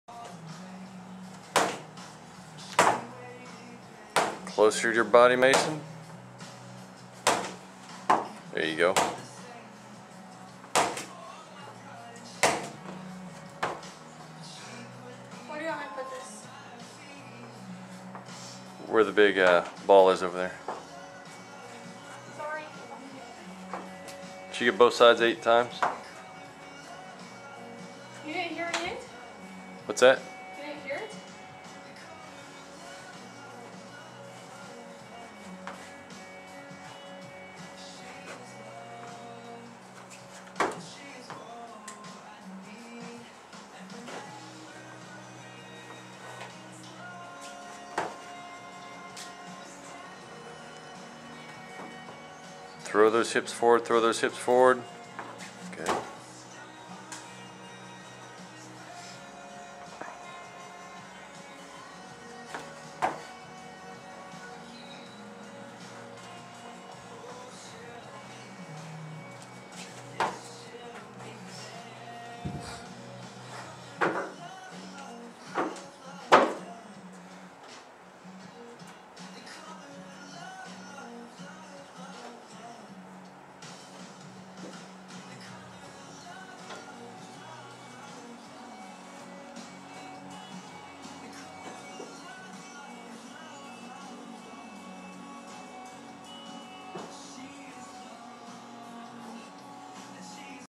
Pinnacle Athletes Single Arm Snatches
Pinnacle Performance and Fitness working on total body power during our Summer Sports Performance Enhancement Camp.